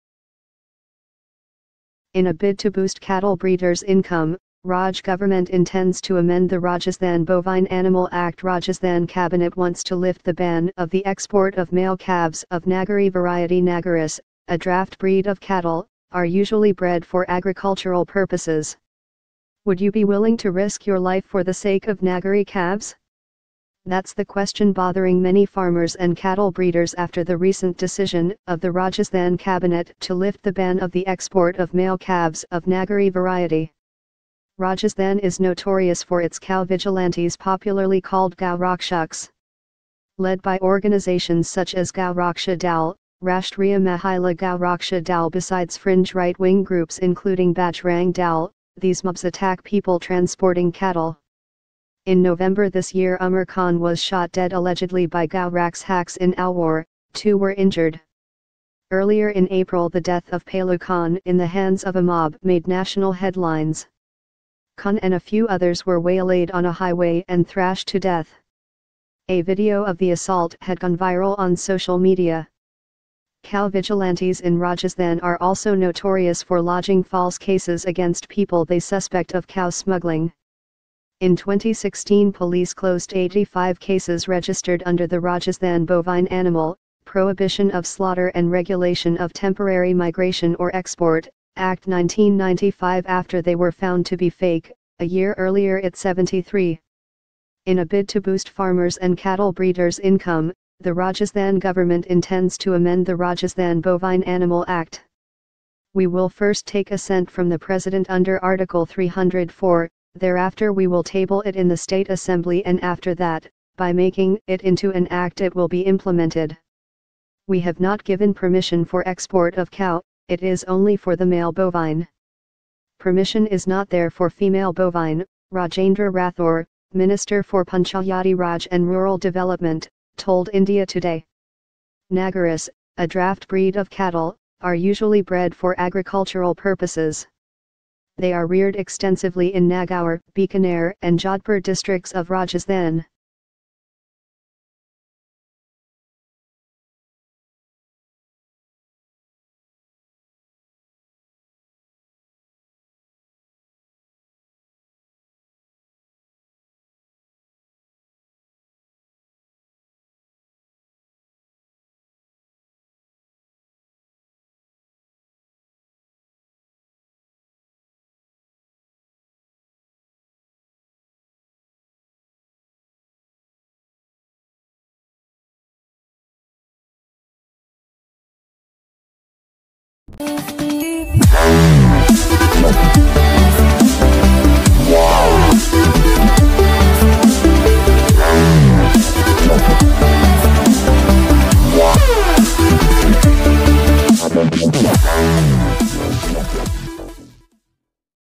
In the land of gau rakshaks, Rajasthan wants to export calves
In a bid to boost cattle breeders' income, Raje govt intends to amend the Rajasthan Bovine Animal Act.Rajasthan cabinet wants to lift the ban of the export of male calves of Nagori variety.Nagoris, a draught breed of cattle, are usually bred for agricultural purposes.
Would you be willing to risk your life for the sake of Nagori calves? That's the question bothering many farmers and cattle breeders after the recent decision of the Rajasthan cabinet to lift the ban of the export of male calves of Nagori variety.
Rajasthan is notorious for its cow vigilantes popularly called 'gau rakshaks'. Led by organisations such as Gau Raksha Dal, Rashtriya Mahila Gau Raksha Dal besides fringe right wing groups including Bajrang Dal, these mobs attack people transporting cattle.
In November this year Ummar Khan was shot dead allegedly by gau rakshaks in Alwar; two were injured.